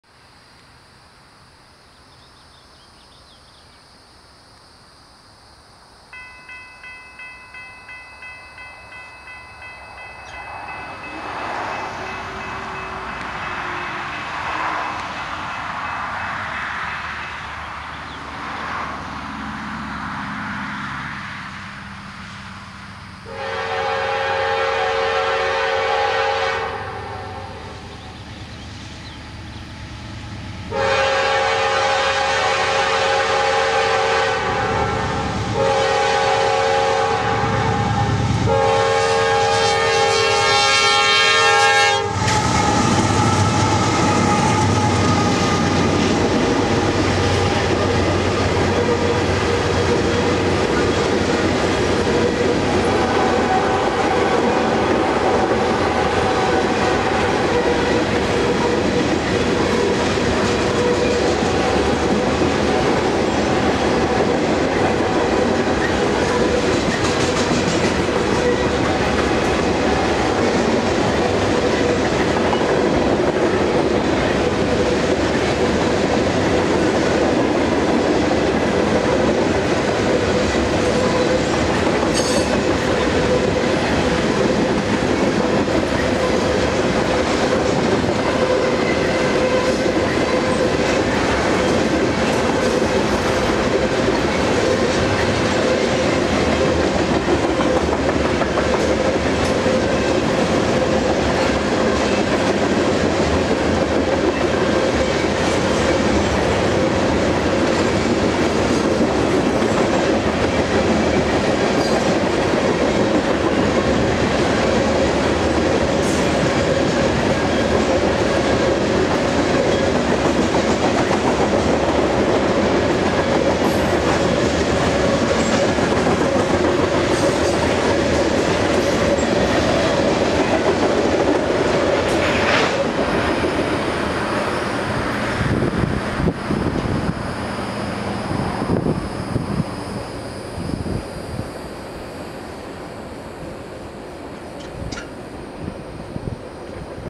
NS 4456 Leads Southbound Loaded Grain Train At Blythewood SC
Southbound Norfolk Southern grain train bound for Monetta SC heads southbound on the NS R Line approaching Blythewood SC at the R87.5 milepost with NS 4456 NS 4237 and NS 9768 as the power.